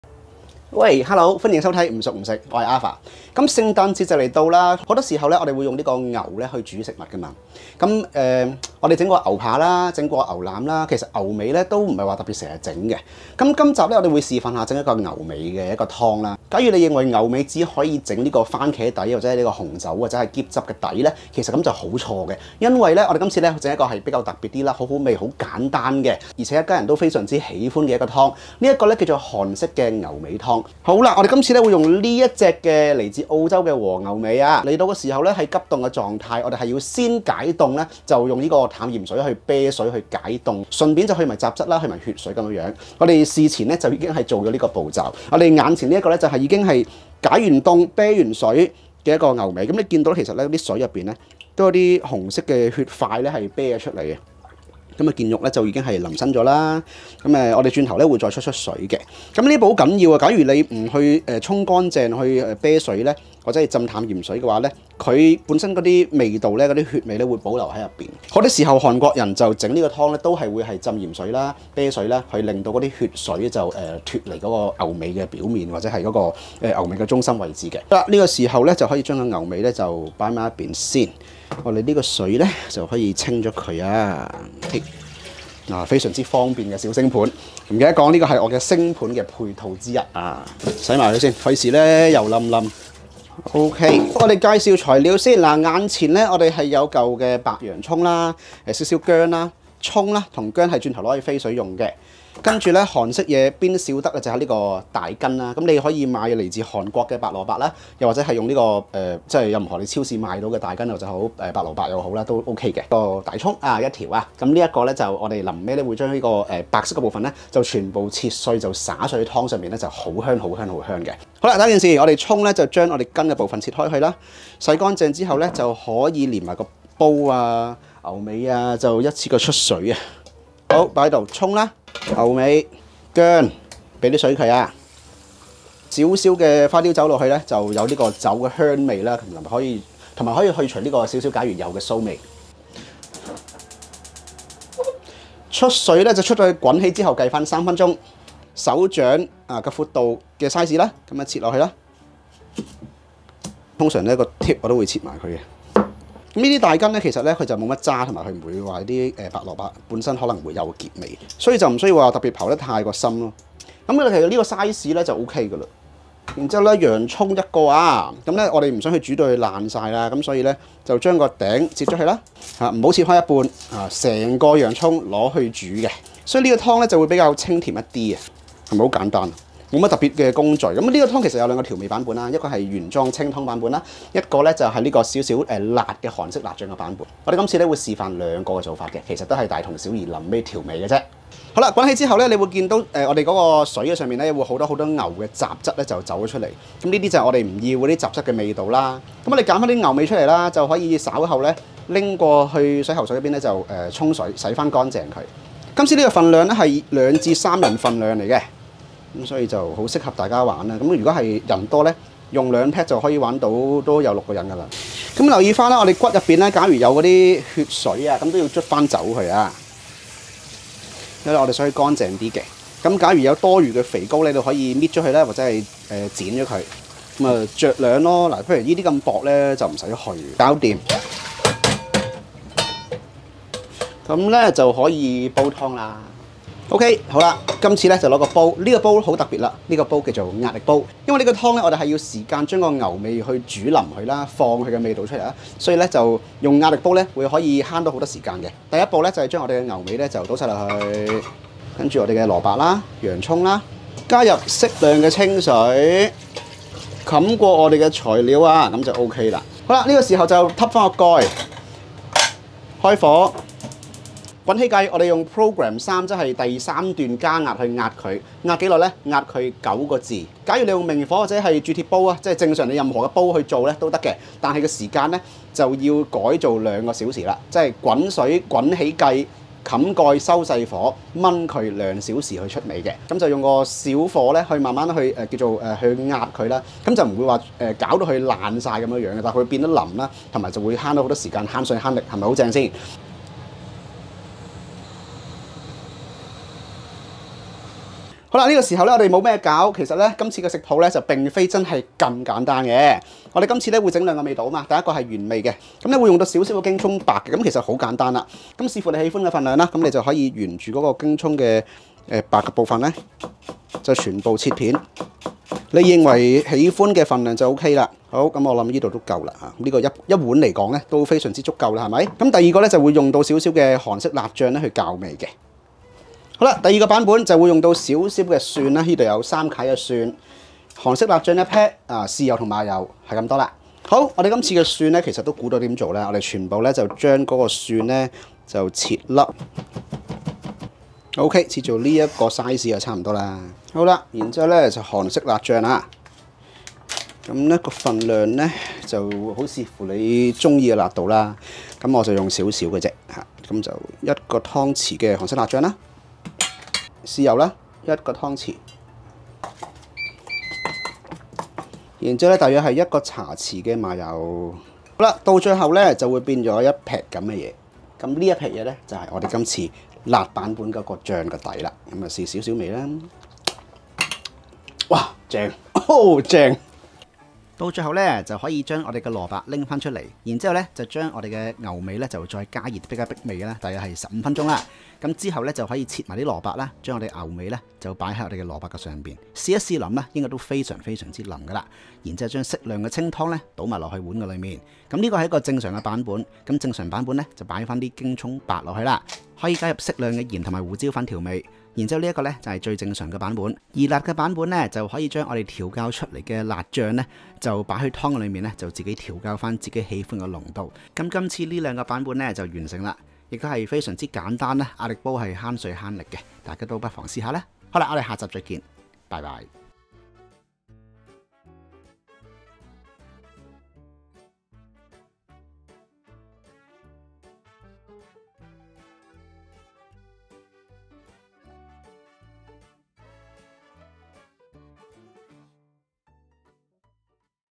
【韓式牛尾湯】清湯 辛辣 2個版本 （中文字幕）Korean Oxtail Soup in 2 Versions
牛尾做法唔多，不過呢個韓式做法可能大家會少啲做。幾清甜，亦都好簡單。
今次示範2個版本，大家隨便調教口味啦！

Ckingroom